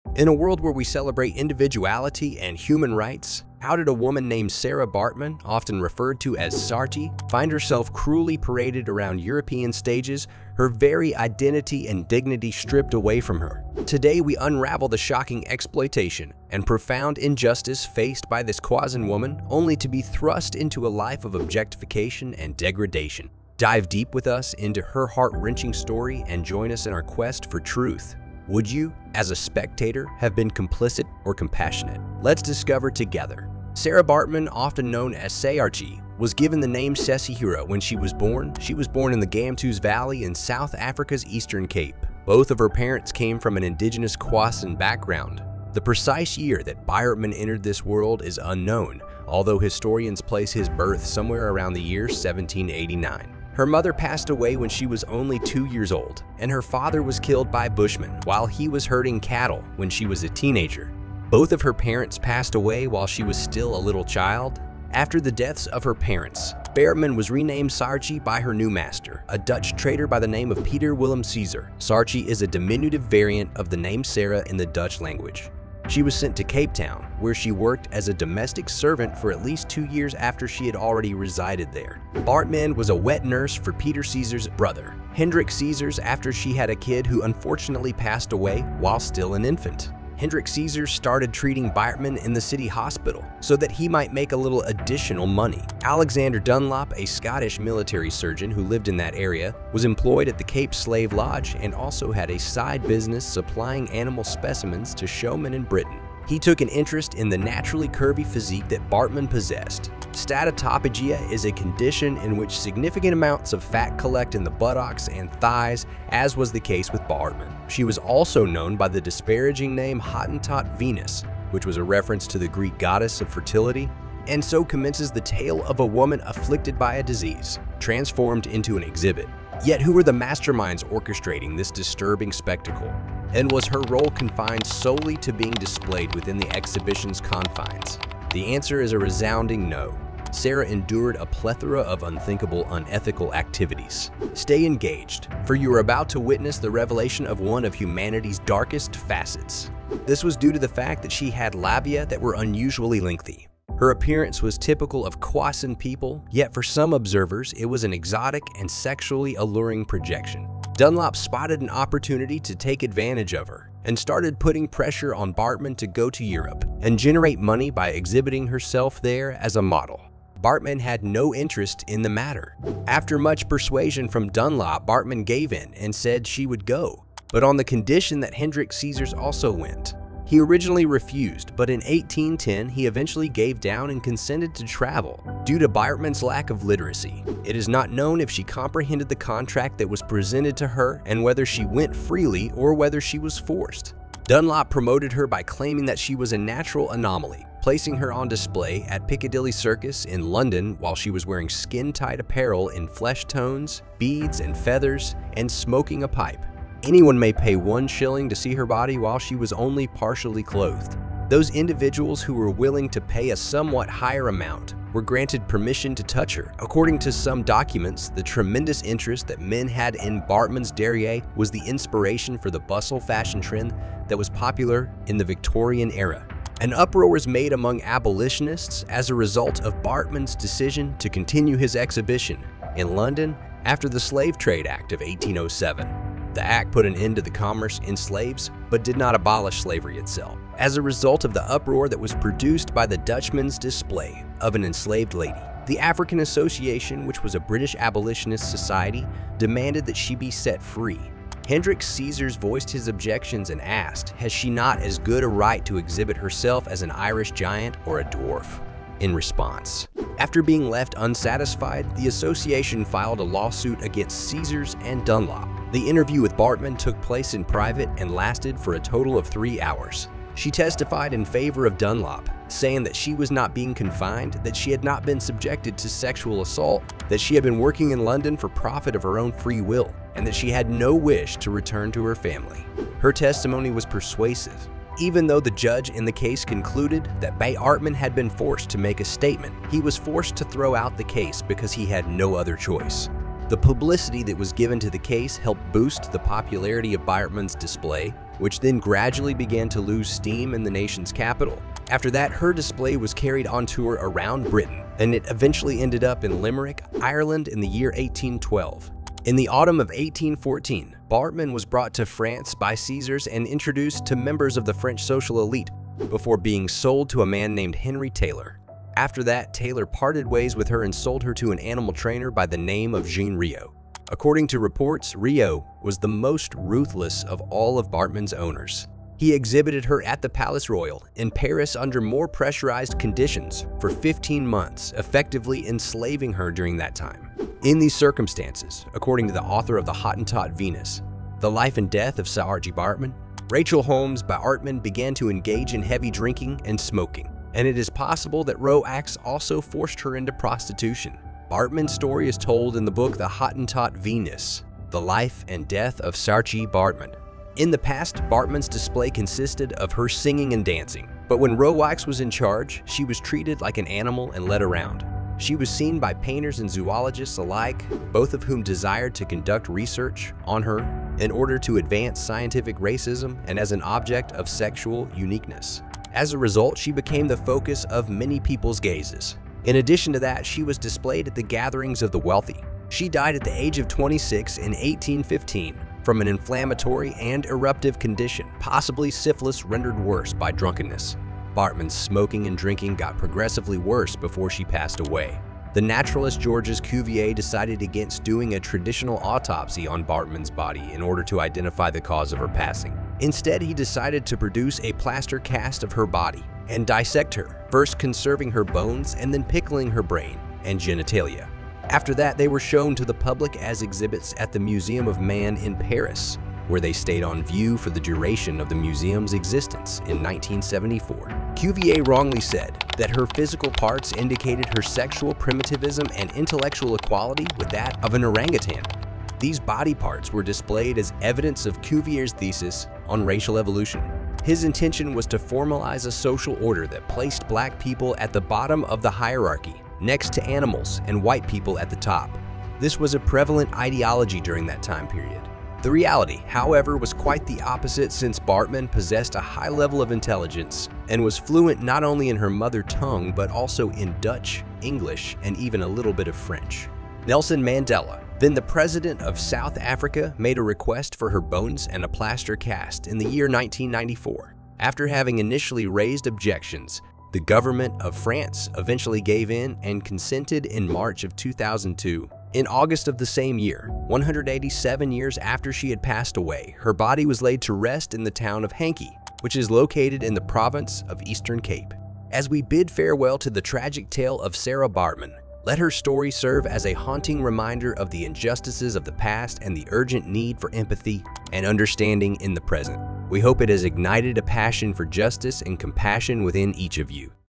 Revealing the Shocking Exploitation and Injustice in Sarah Baartman's Story
In a world where we celebrate individuality and human rights, how did a woman named Sarah Baartman, often referred to as Saartjie, find herself cruelly paraded around European stages, her very identity and dignity stripped away from her? Today, we unravel the shocking exploitation and profound injustice faced by this Khoisan woman, only to be thrust into a life of objectification and degradation. Dive deep with us into her heart-wrenching story and join us in our quest for truth. Would you, as a spectator, have been complicit or compassionate? Let's discover together.

Our channel is compliant with YouTube's Fair Use Policy as we create a storyline and cut down several hours of long footage to a few minutes to bring out the most entertaining and educational parts.